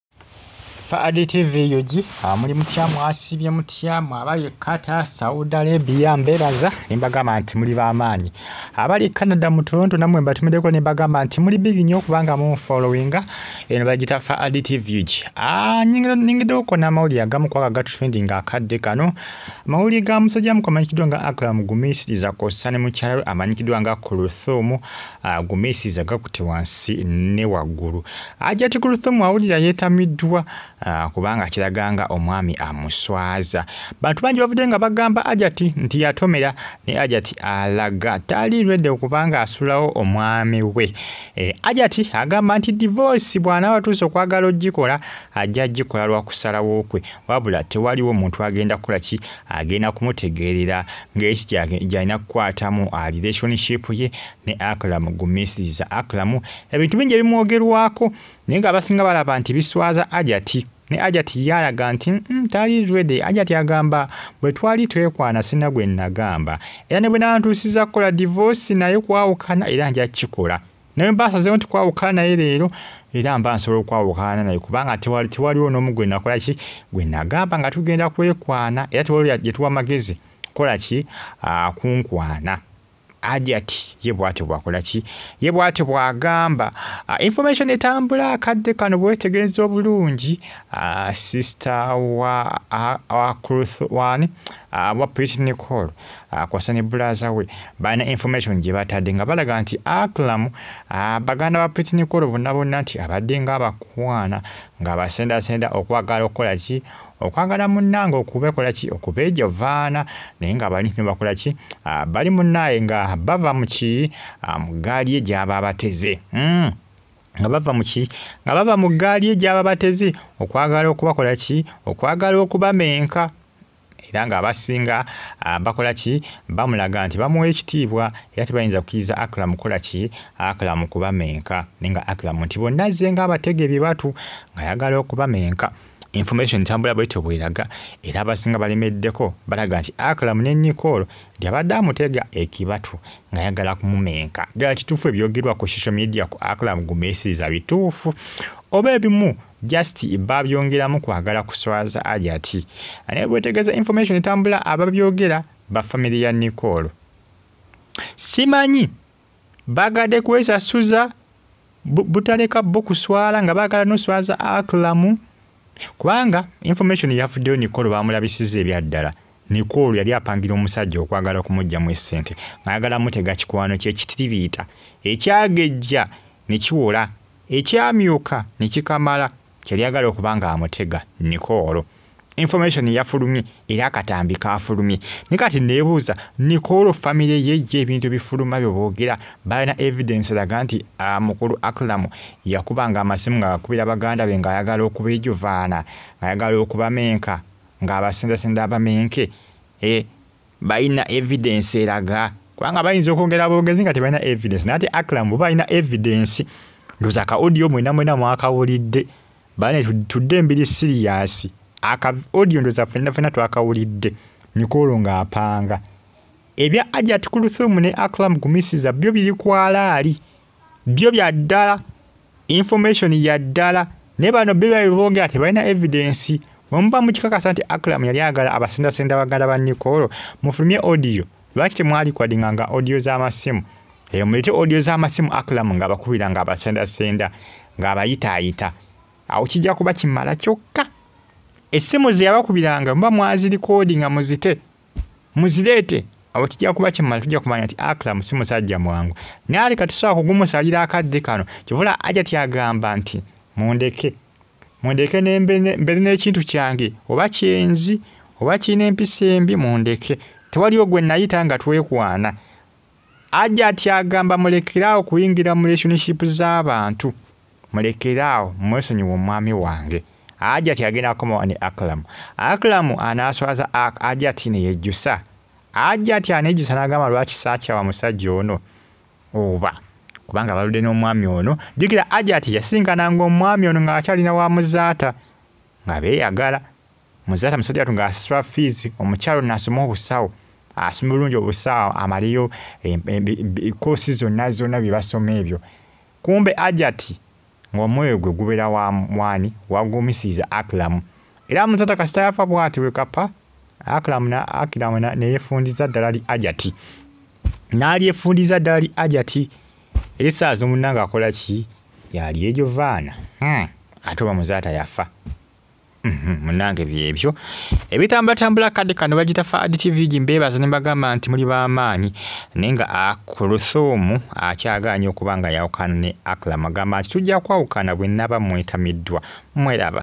Finally 🙆 Hajjat Kuluthumu Akozze Devorce Ne Acram Gumisiliza Bawukannye Ebyadala Bibiino 🥺
Hajjat Kuluthumu Akozze Divorce Ne Acram Gumisiliza (@FAHAD TV UG)